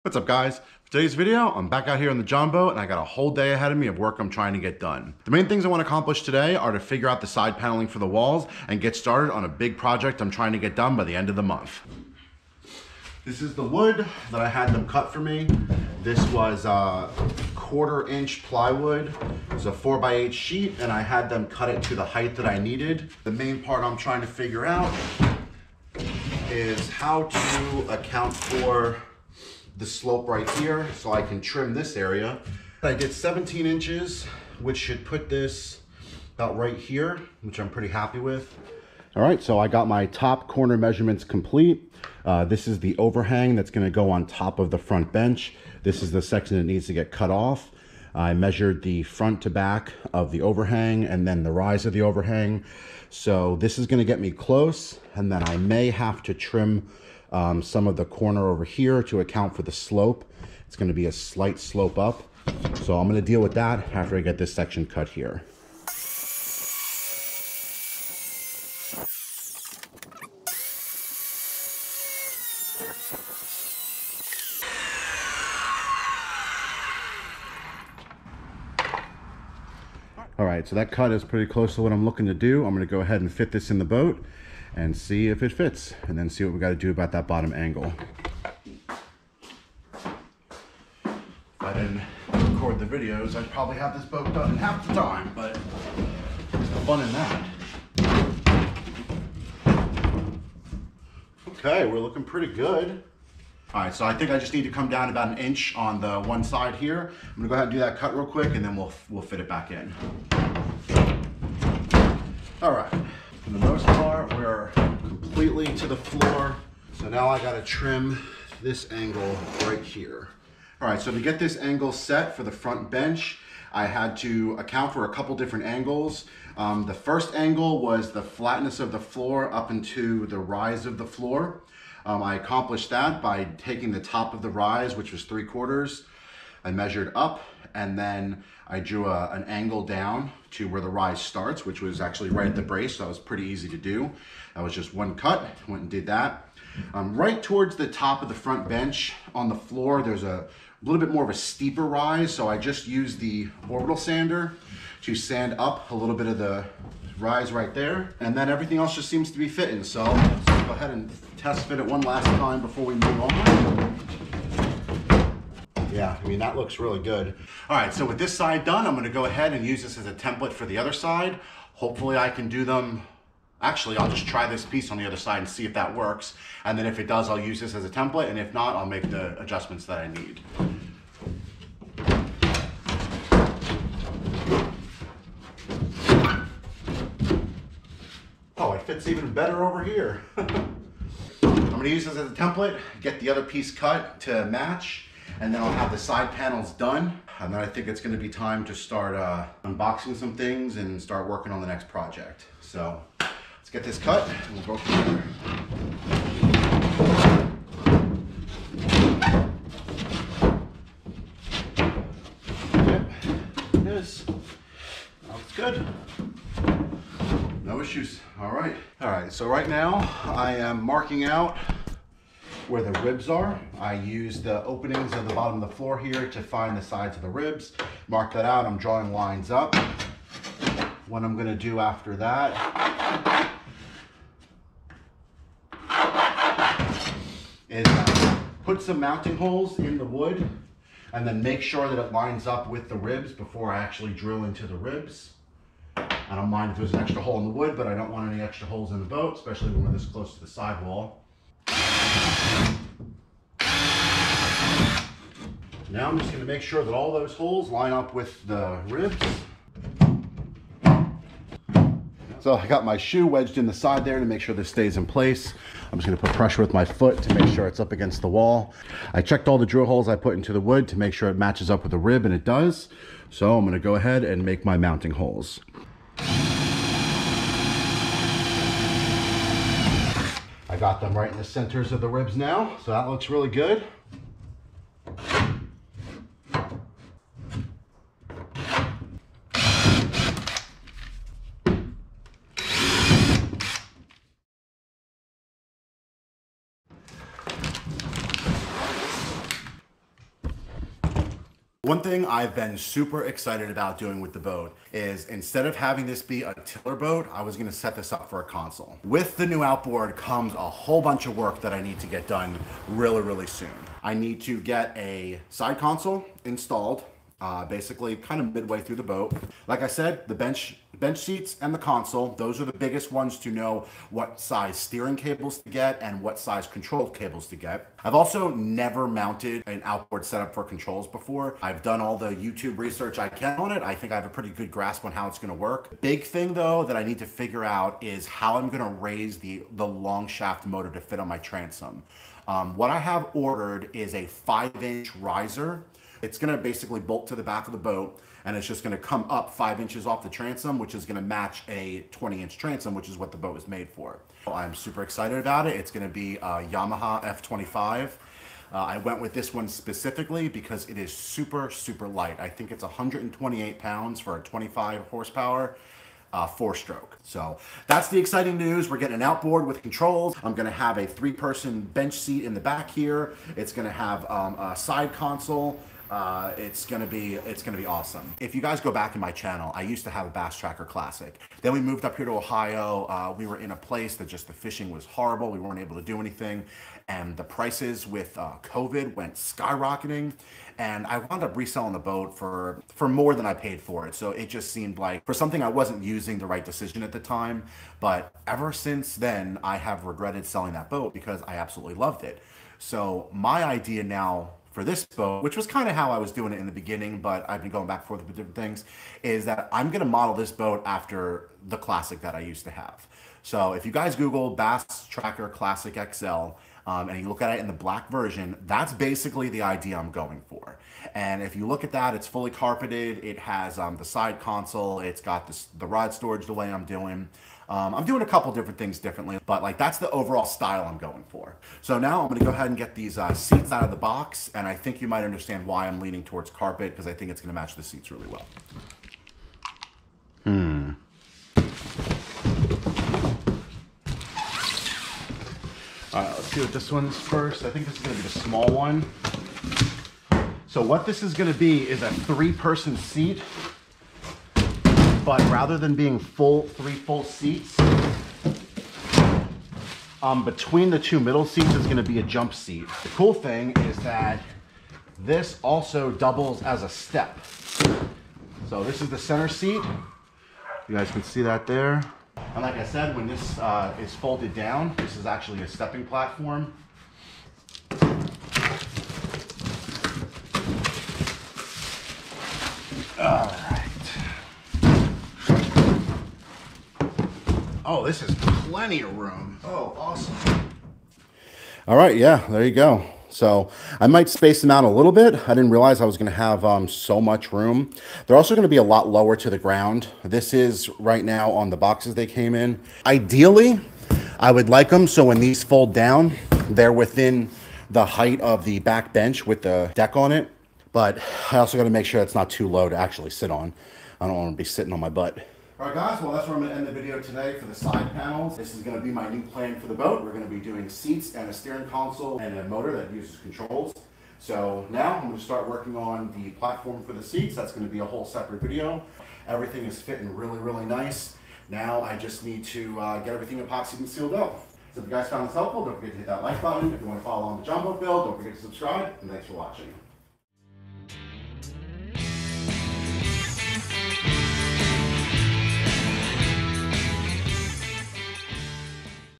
DIY Boat Upgrade: How to Add Side Paneling to your Jon Boat Build
- In this video, I show you step-by-step how to give your jon boat a stylish upgrade by adding side paneling. This easy DIY project will not only improve the look of your boat but also add extra storage space. We'll go through the materials you'll need, the tools, and the process of cutting, measuring, and attaching the paneling to your boat. Whether you're an experienced boater or a beginner, you'll find this video easy to follow and understand. Upgrade your jon boat to make it even better out on the water with this simple but effective DIY project.

Part 2 Video - Coming soon!